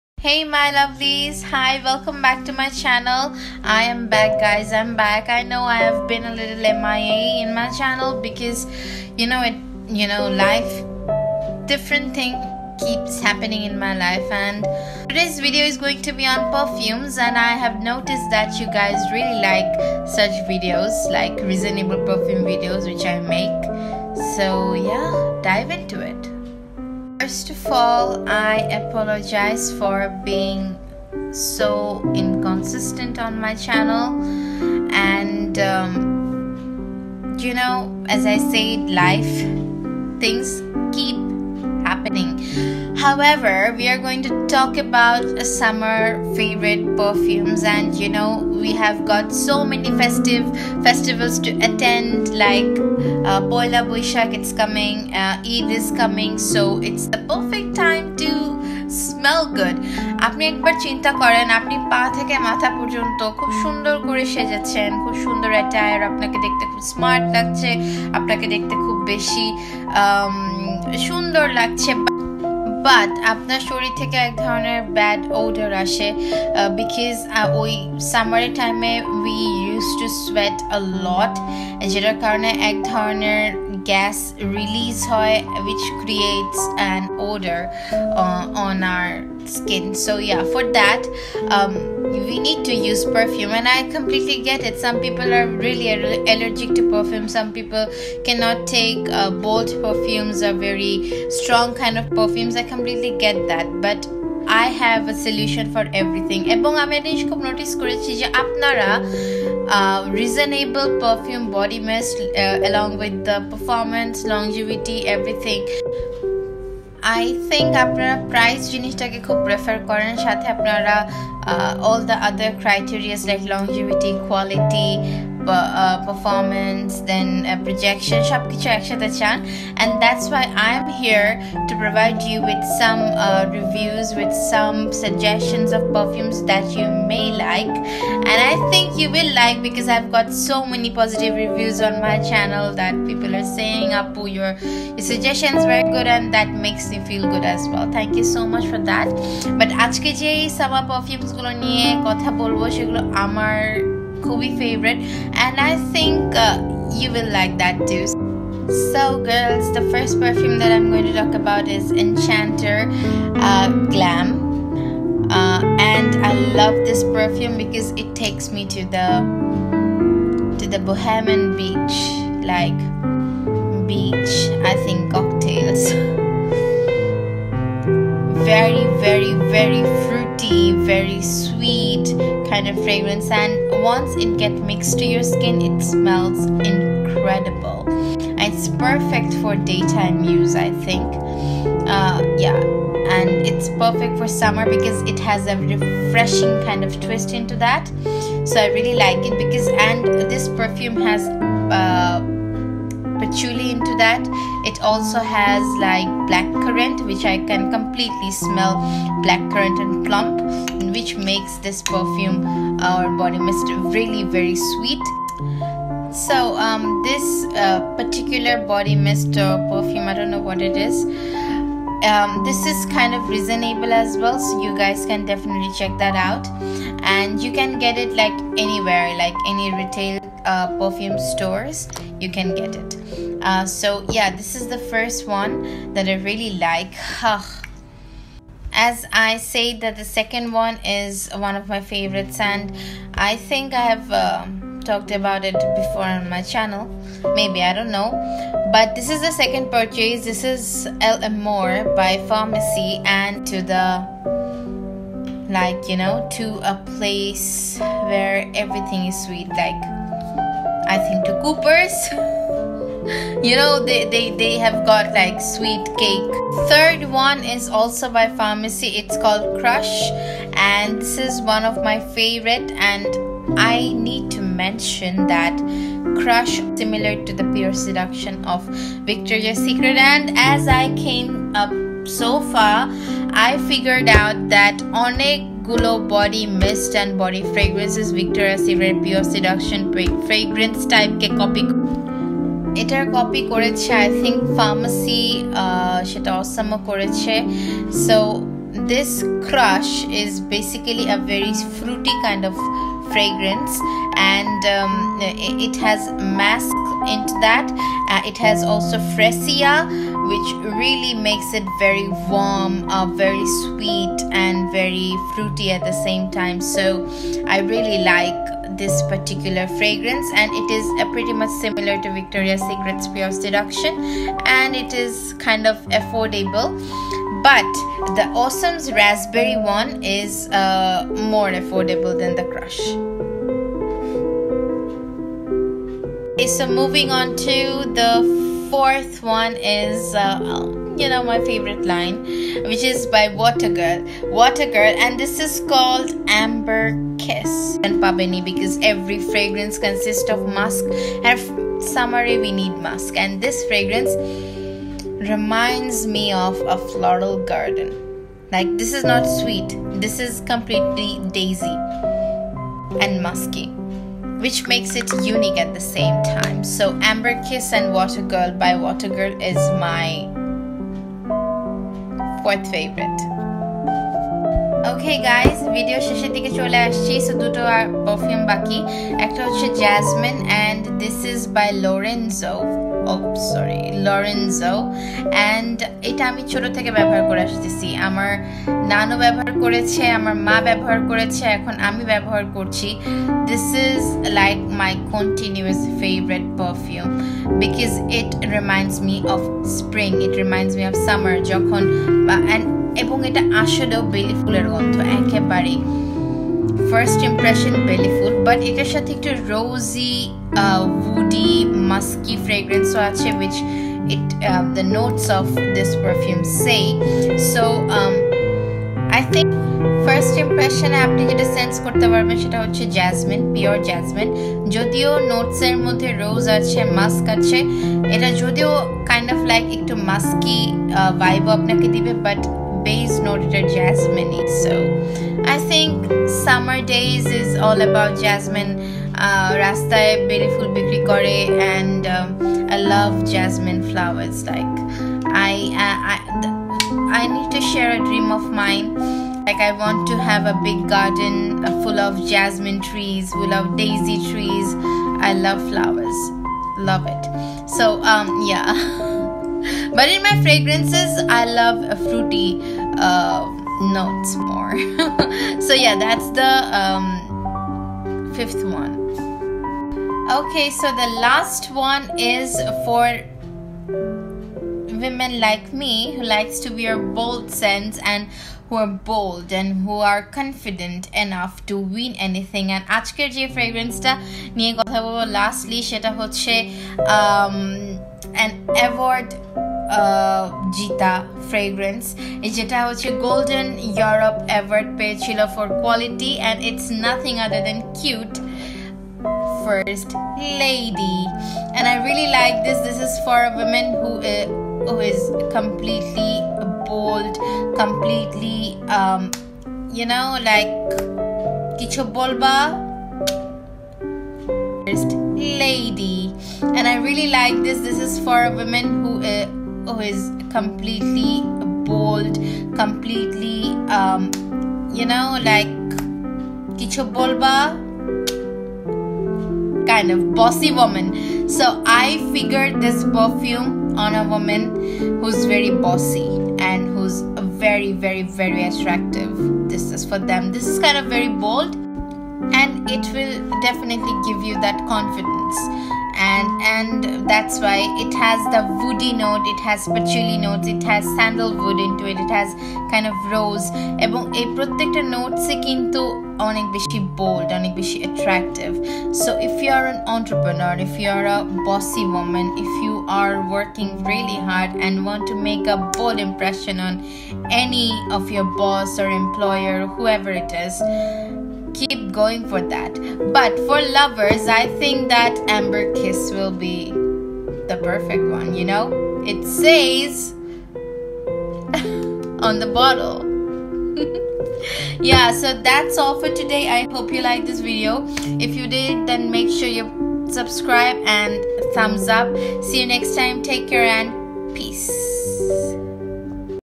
Expensive smelling fragrances at an affordable rate! 😱 My summer favourite fragrances ❤️
Ramadan Mubarak to all of you. I hope you will find my video helpful.
A bog surprise is waiting which I am going to announce on my next video and for that update, keep your eyes on my channel.